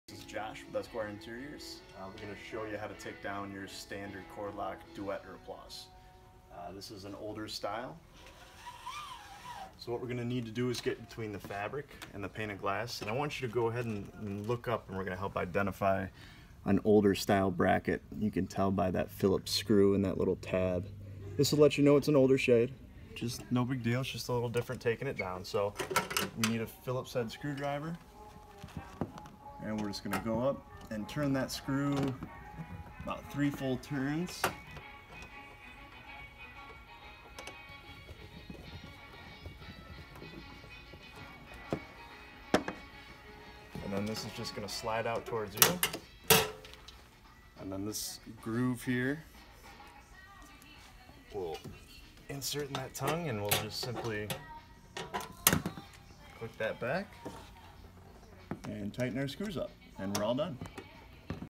remove Standard cordlock Duette
How to remove and re install older style duette